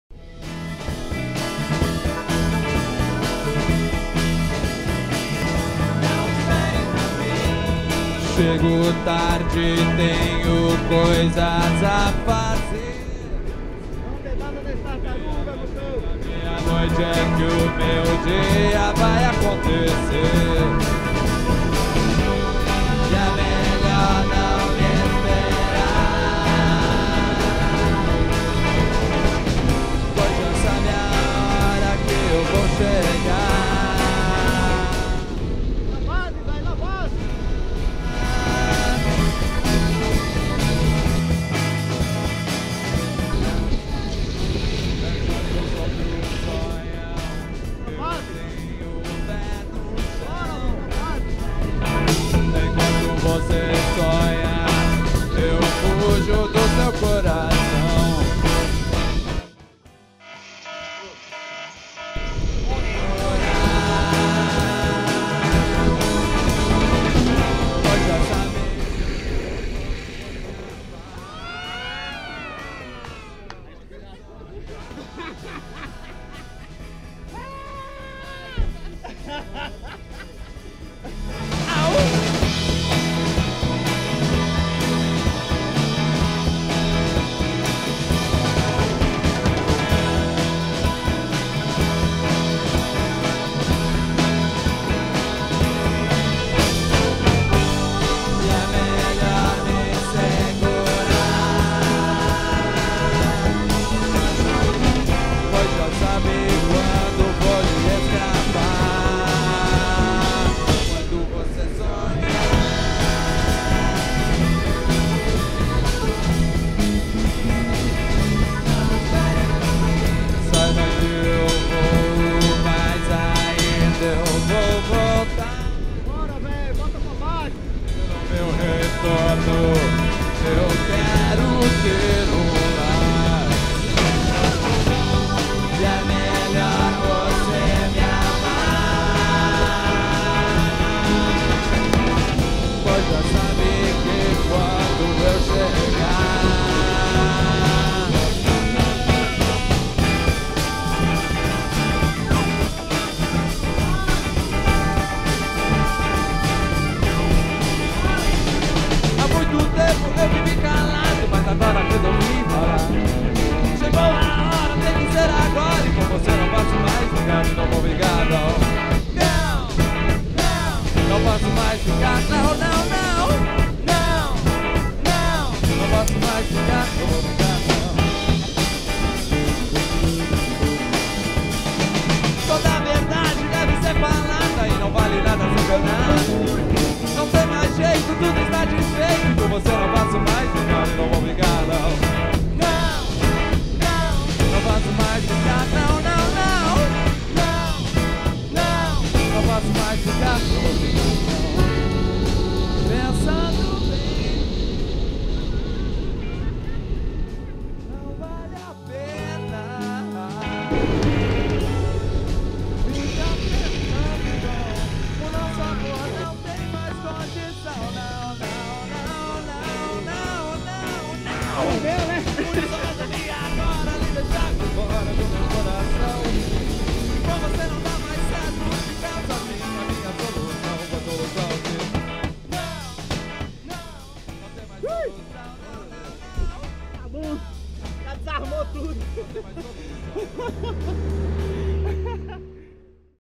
Treino de Freeride Flying Satans
Data: 20/04/2015
Local: Drop do Casquinha/Despenco/SP
Riders:
Sasha Reis
Guto Negão

Descrição
Treino e slides no casquinha com direito a sentido aranha e trolagem básica no Veio.

Use sempre um capacete e as demais proteções. Skate Safe sempre!